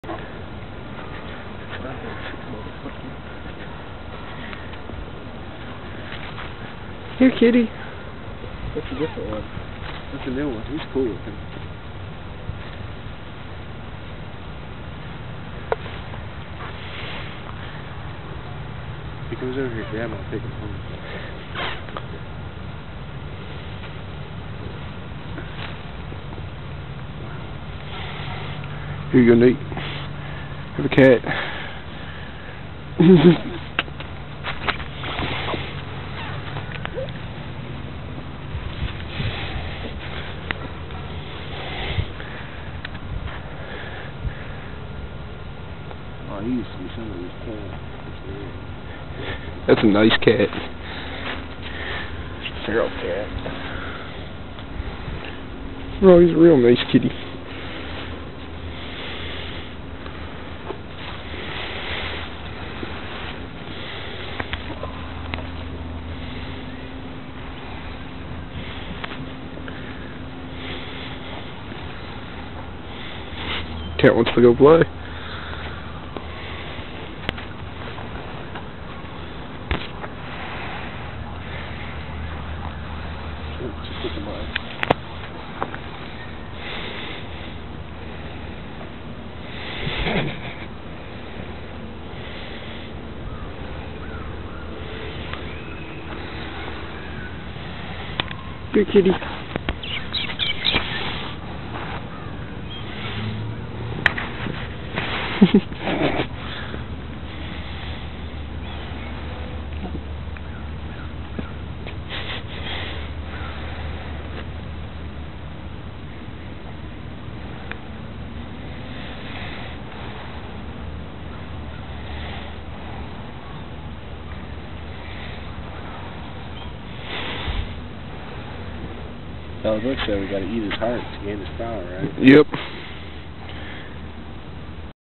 awesome cat from the Cat Village
this was the nicest cat in the village, makes me think he is an abandoned pet... he followed us around for an hour, then bolted off out of nowhere and almost got nailed by a van, then dissapeared into the bushes